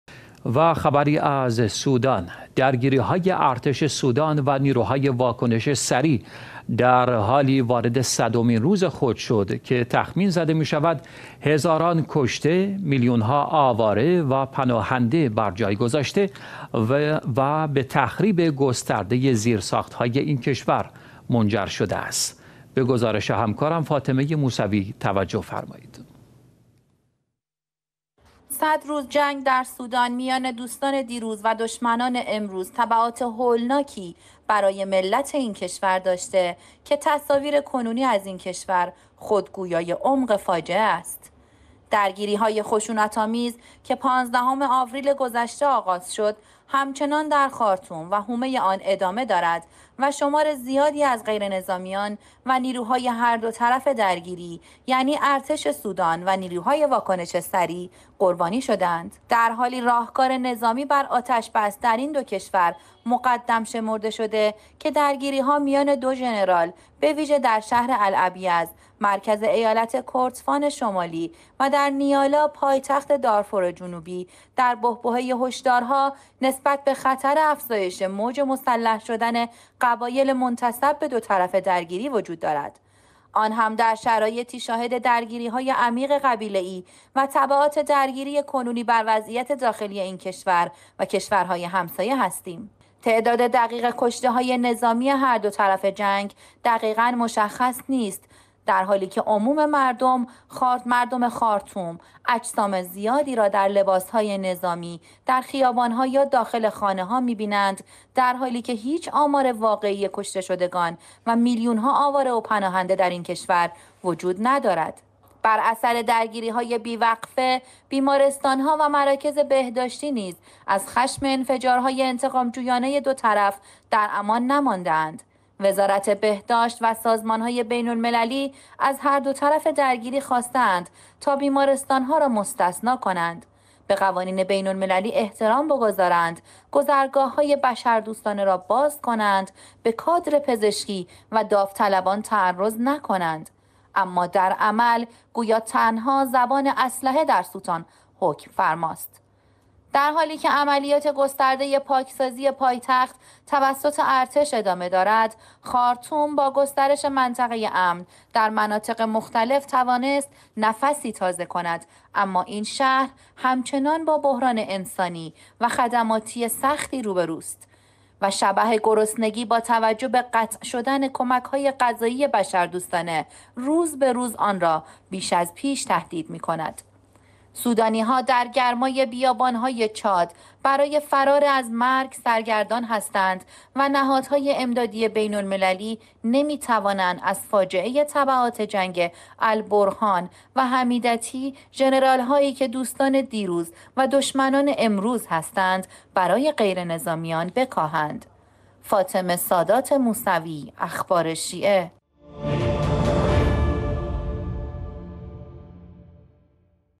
سودان؛ 100 روز جنگ بین دوستان دیروز و دشمنان امروز، فاطمه موسوی گزارش می دهد
درگیری‌های ارتش سودان و نیروهای واکنش سریع در حالی وارد صدمین روز خود شد که تخمین زده می شود هزاران کشته؛ میلیون ها آواره و پناهنده برجای گذاشته و به تخریب گسترده زیرساخت‌های این کشور منجر شده است.

صد روز جنگ در سودان، میان دوستان دیروز و دشمنان امروز تبعات هولناکی برای ملت این کشور داشته که تصاویر کنونی از این کشور خود گویای عمق فاجعه است.
درگیری‌های خشونت‌آمیز که پانزدهم آوریل گذشته آغاز شد، همچنان در خارطوم و حومه آن ادامه دارد و شمار زیادی از غیرنظامیان و نیروهای هر دو طرف درگیری، یعنی ارتش سودان و نیروهای واکنش سریع، قربانی شده اند.
در حالی راهکار نظامی بر آتش‌بس‌ در این کشور مقدم شمرده شده که درگیری‌ها میان دو ژنرال، به‌ویژه در شهر الابیض ، مرکز ایالت کردفان شمالی، و در نیالا، پایتخت دارفور جنوبی، در بحبوحه هشدارها نسبت به خطر افزایش موج مسلح شدن قبایل منتسب به دو طرف درگیری وجود دارد. آنهم در شرایطی شاهد درگیری های عمیق قبیله ای و تبعات درگیری کنونی بر وضعیت داخلی این کشور و کشورهای همسایه هستیم.
تعداد دقیق کشته‌ های نظامی هر دو طرف جنگ دقیقاً مشخص نیست، در حالی که عموم مردم خارطوم اجساد زیادی را در لباس‌های نظامی، در خیابان‌ها یا داخل خانه‌ها می‌بینند در حالی که هیچ آمار واقعی کشته‌شدگان و میلیون ها آواره و پناهنده در این کشور وجود ندارد.
بر اثر درگیری‌های بی‌وقفه، بیمارستان‌ها و مراکز بهداشتی نیز از خشم انفجارهای انتقام‌جویانه دو طرف در امان نمانده است، وزارت بهداشت و سازمان‌های بین‌المللی از هر دو طرف درگیر خواسته اند تا بیمارستان‌ها را مستثنی کنند، به قوانین بین‌المللی احترام بگذارند، گذرگاههای بشردوستانه را باز کنند، به کادر پزشکی و داوطلبان تعرض نکنند؛ اما در عمل گویا تنها زبان اسلحه در سودان حکم فرماست.
در حالی که عملیات گسترده پاکسازی پایتخت توسط ارتش ادامه دارد، خارطوم با گسترش منطقه امن در مناطق مختلف توانست نفسی تازه کند، اما این شهر همچنان با بحران انسانی و خدماتی سختی روبه‌رو است و شبح گرسنگی با توجه به قطع شدن کمک‌های غذایی بشردوستانه، روز به روز آن را بیش از پیش تهدید می‌کند.
سودانی‌ها در گرمای بیابان های چاد، برای فرار از مرگ سرگردان هستند و نهادهای امدادی بین‌المللی نمی‌توانند از فاجعه تبعات جنگ البرهان و حمیدتی ژنرال هایی که دوستان دیروز و دشمنان امروز هستند؛ برای غیرنظامیان بکاهند.